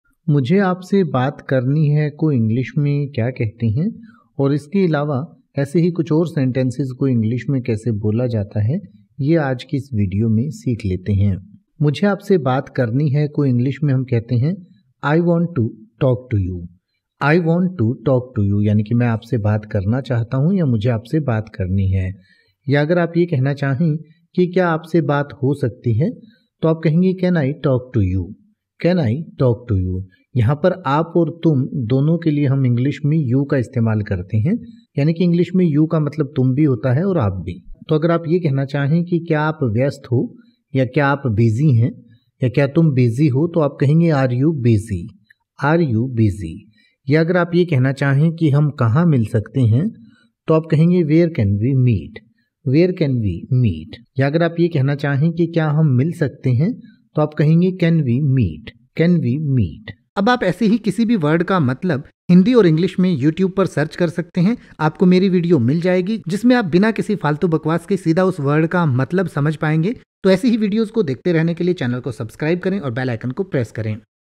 मुझे आपसे बात करनी हैं को इंग्लिश में क्या कहते हैं | mujhe aapse baat karni hai in english
इस वीडियो में आप मुझे आपसे बात करनी हैं का हिंदी में मतलब समझेंगे और साथ में मुझे आपसे बात करनी हैं का pronunciation भी सीखेंगें यानि की आप मुझे आपसे बात करनी हैं के अर्थ के साथ साथ ये भी सीखेंगें की मुझे आपसे बात करनी हैं को कैसे बोले या मुझे आपसे बात करनी हैं को बोलने का सही तरीका क्या है |

तो बस एक मिनट में सीखिए मुझे आपसे बात करनी हैं को|
Let’s learn Hindi Meaning of mujhe aapse baat karni hai in english in detail.

मुझे आपसे बात करनी हैं का अंग्रेजी में अर्थ होता है
I want to talk to you.
मुझे आपसे बात करनी हैं।
Can I talk to you?
क्या आपसे बात हो सकती है?
Are  you busy.
क्या आप वयस्त हो।
Where can we meet?
हम कहां मिल सकते हैं?
can we meet?
क्या हम मिल सकते हैं?